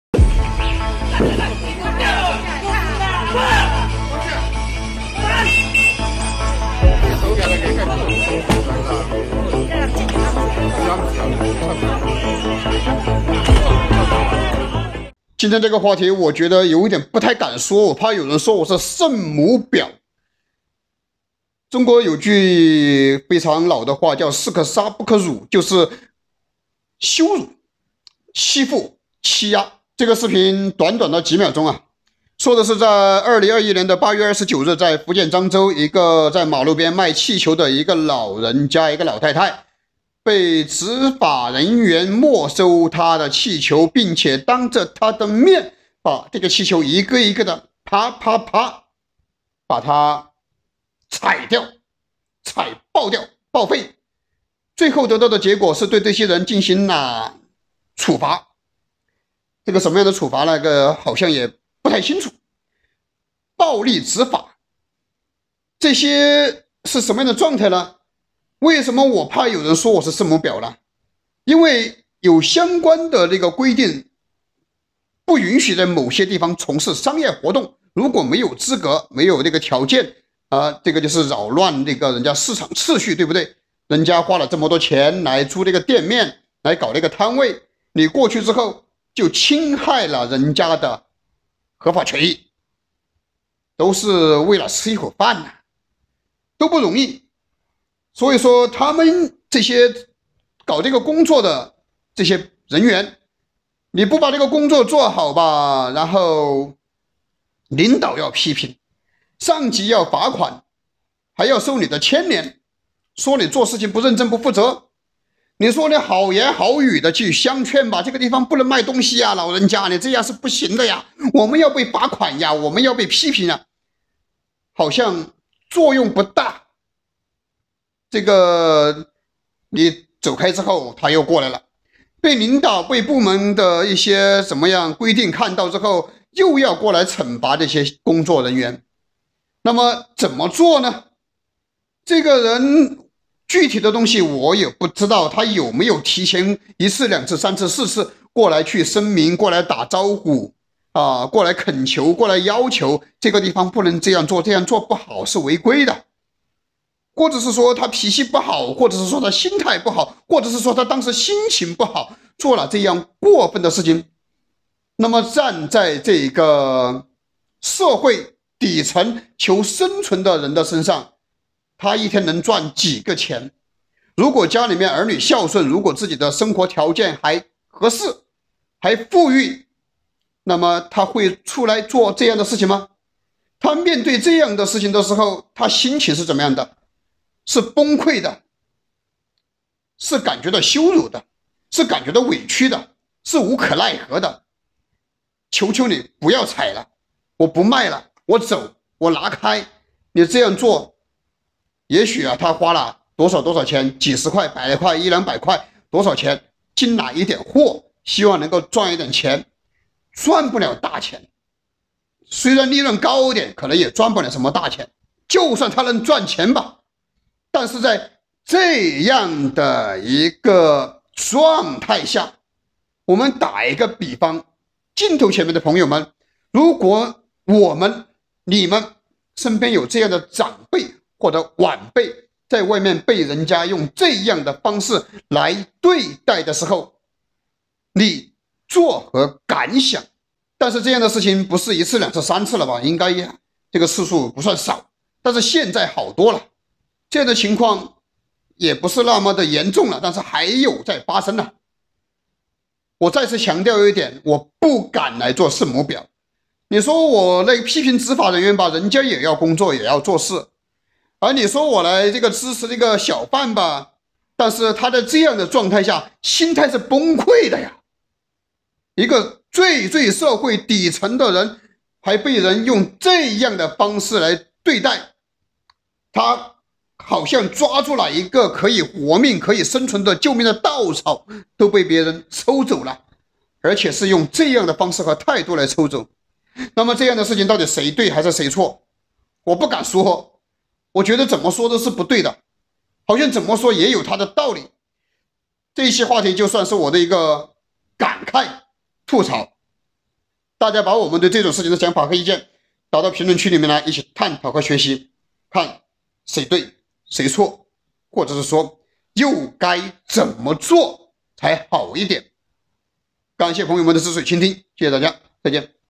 8月29日，福建漳州一個六旬賣氣球老人，遭城管粗暴執法，氣球被踩爆！老人哭喊“不賣了”，是規矩重要還是生存重要？粗暴執法
8月29日，福建漳州一個六旬賣氣球老人，遭城管粗暴執法，氣球被踩爆！老人哭喊“不賣了”，是規矩重要還是生存重要？